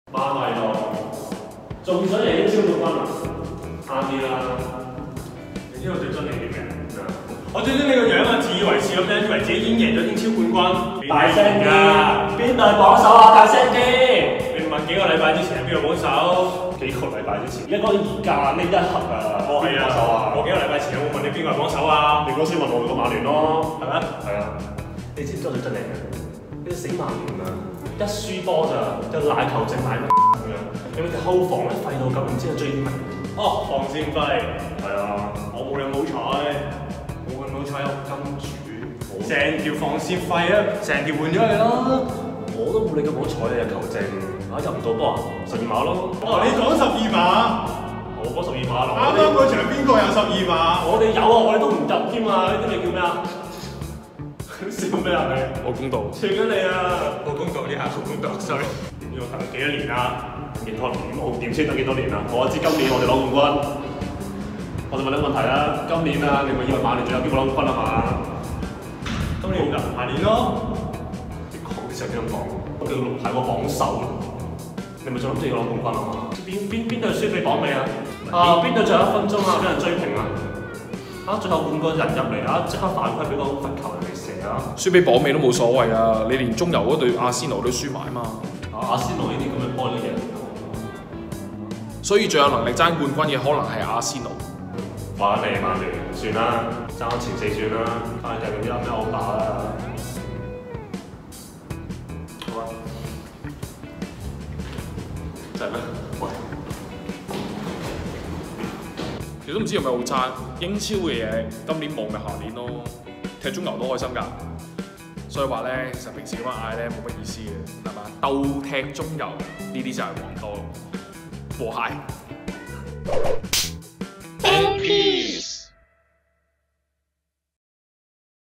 【英超Review Week22+23 — 重挫曼迷銳氣示範】
早前曾登上榜首嘅曼聯，由於接連失分，再次被同市宿敵曼城拉開分差。曼迷 @隆仔2.0 不甘示弱，與曼城迷 @ArLok 發生口角，其銳氣更遭到重挫.

幸而最後由中游份子列斯聯球迷（前阿迷）@檸檬 Lemon Ho 擺平事件，和氣收場。雙方身體沒有明顯傷痕，唯隆仔內心之傷則不得而知.